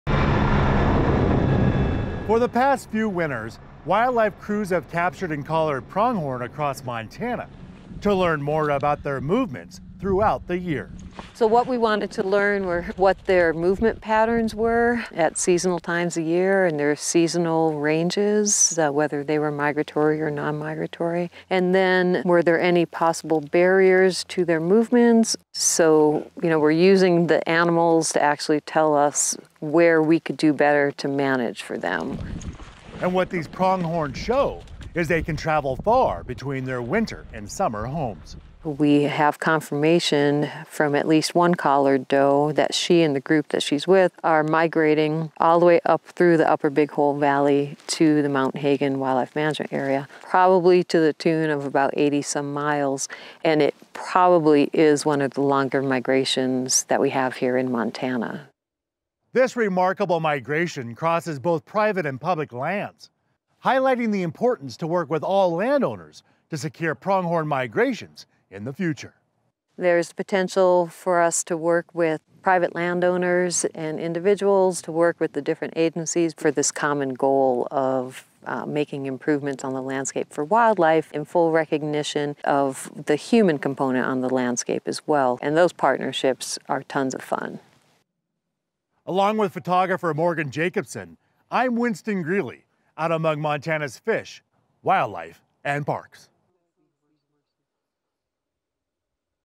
Big Hole Pronghorn
Some surprising findings from one of the largest pronghorn research projects in Montana.